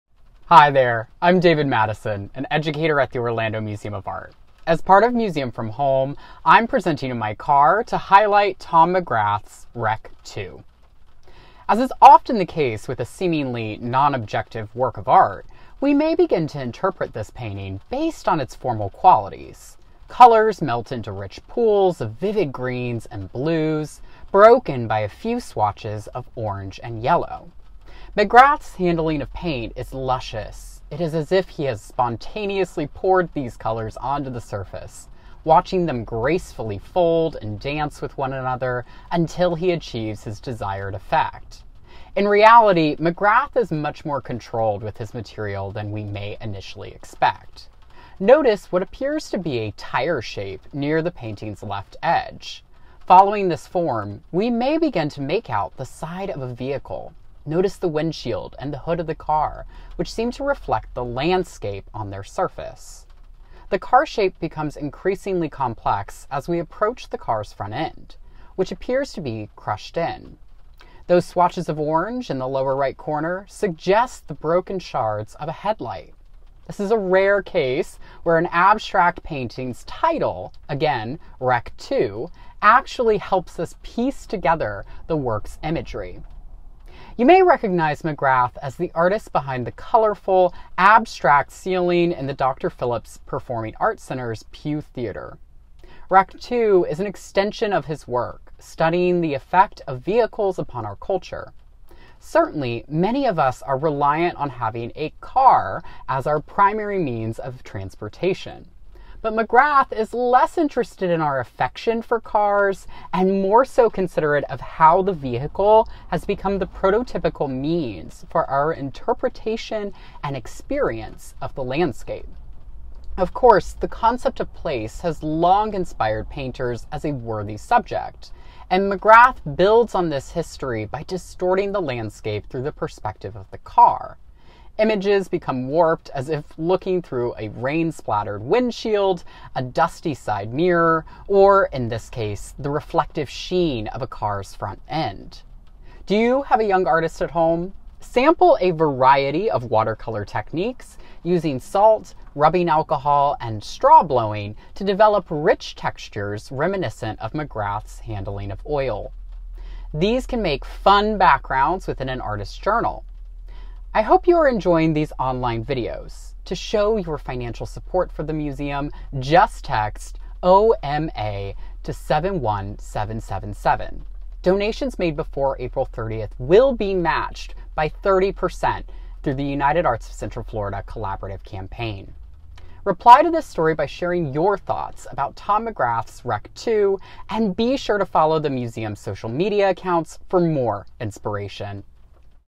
Museum from Home: Tom McGrath's "Wreck 2" (2005)
Learn about a masterpiece from the Orlando Museum of Art!